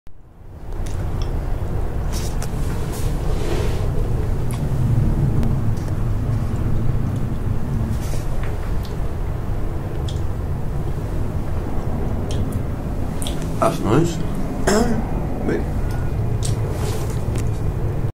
Emirates Air-Line over the O2 Arena, London, at night
a flyover O2 Arena in London from Emirates Air-Line during the night.